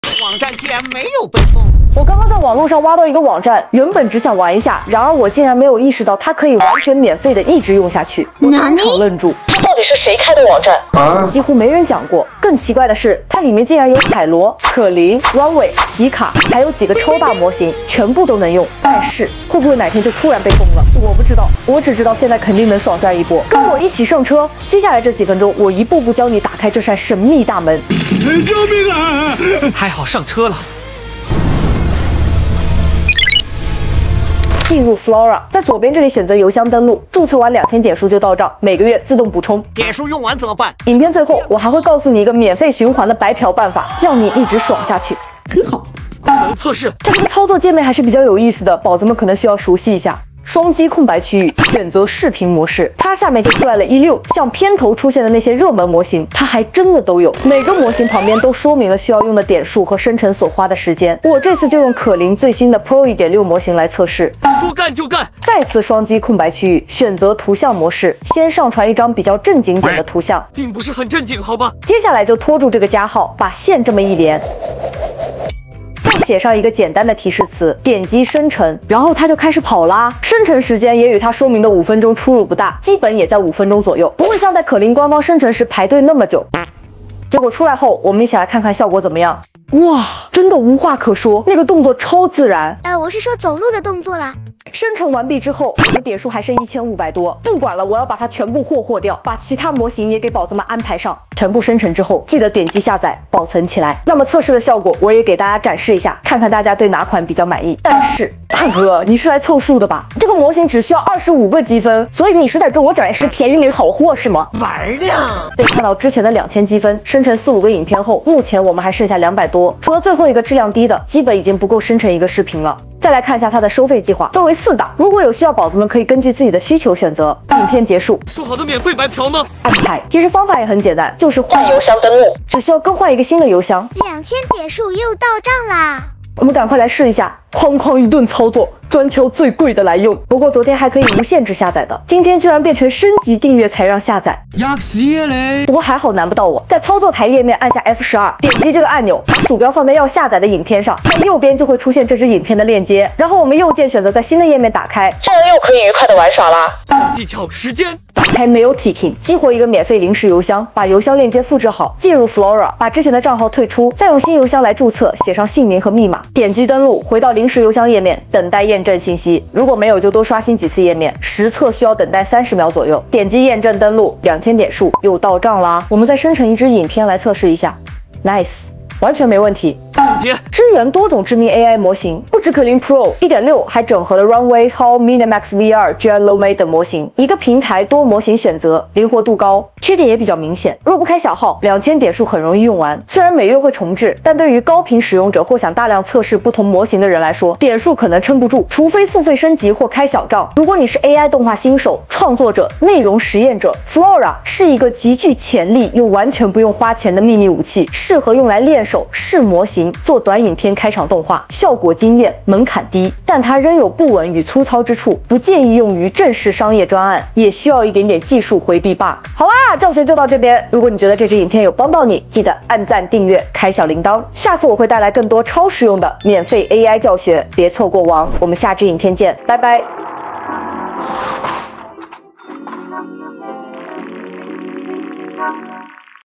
【用一次少一次】沒想到這網站真的能做到！免費用 Kling AI無限生影片，還偷偷內建7大模型🔥免費開啟無限次快速生成功能！
AI影片必備輔助工具，批次增強、升級AI影片HitPaw VikPea：
💥手把手教你如何使用 Kling AI Pro 1.6，免費開啟無限次快速生成功能，打造高質感AI影片。不只這樣，這個神秘平台還暗藏了 7 款超強 AI 模型，包括 Runway、HYO、Minamax V2 等等，通通免費用！

📌 這支影片你會學到：

✅ 如何註冊帳號拿免費點數（每月自動刷新）
✅ 怎麼上傳圖片 → 一鍵變動畫影片
✅ 哪些模型值得一試、效果最驚豔
✅ 破解下載問題、開多帳戶的實用小技巧

⚠️ 如果你是做 YouTube、短影音、AI創作，千萬不要錯過這個寶藏工具！
這麼香的功能會不會哪天突然被鎖起來？我不知道，但現在能用，就該狠狠用爆它！

時間軸：
00:00 這個網站竟然還沒被封？
00:33 又是一個割韭菜的AI工具？
00:46 功能測試
01:28 效果展示
02:08 小技巧
02:20 下載方法
02:38 循環免費
03:00 優缺點總結

免費使用ChatGPT-4.0機器人